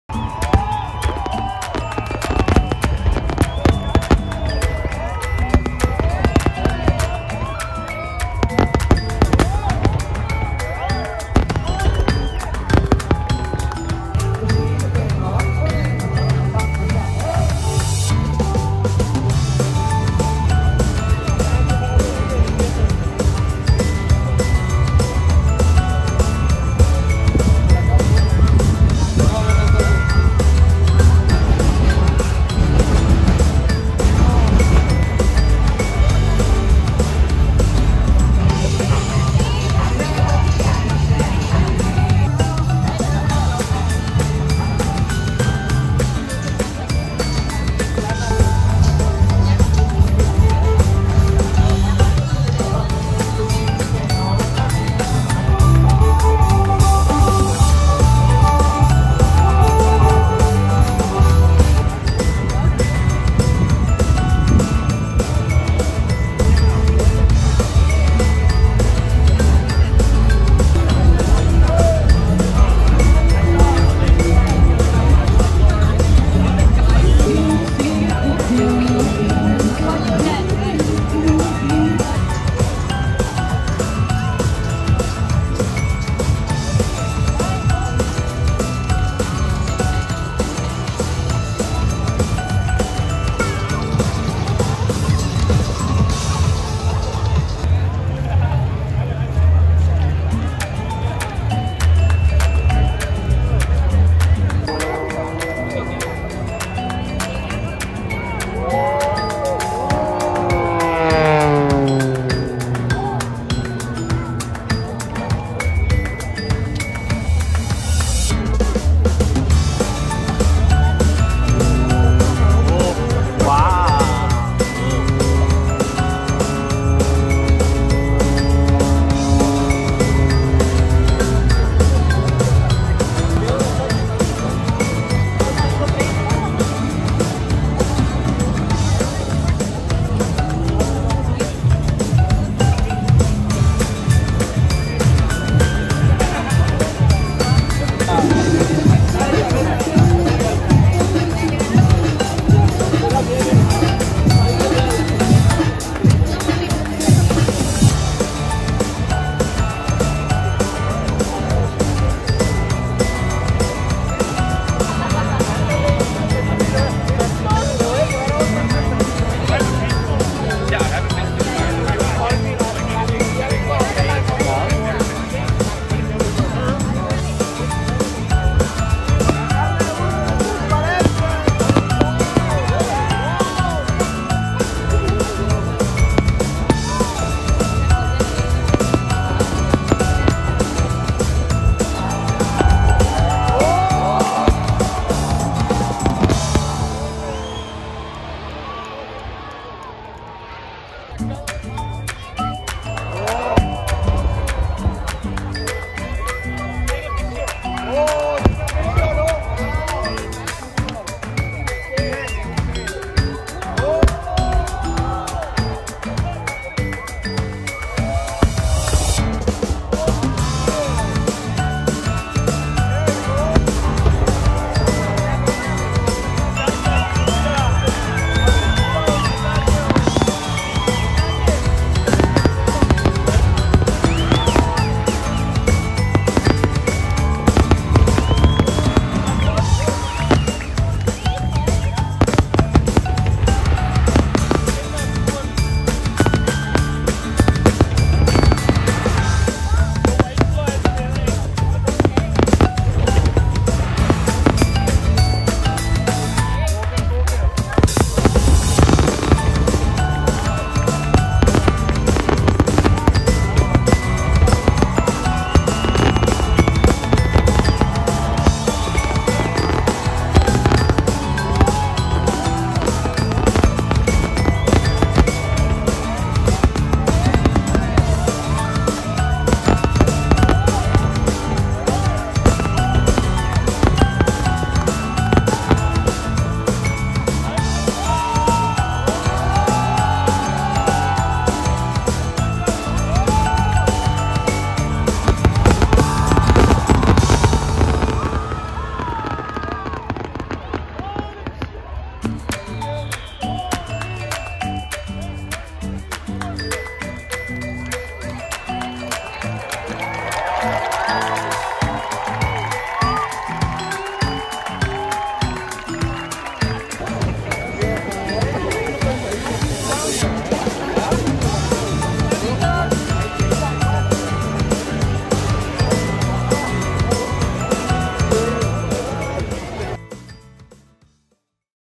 【ダイジェスト】バンクーバーの花火大会で日本の花火。イングリッシュベイの様子 / Celebration of Light at English Bay 2022
2022年7月23日（土）、バンクーバーの花火大会 Celebration of Light が3年ぶりに開催となりました。

6pm頃からEnglish Bayの様子を動画に収めたので、会場の雰囲気をチェックしてみてください。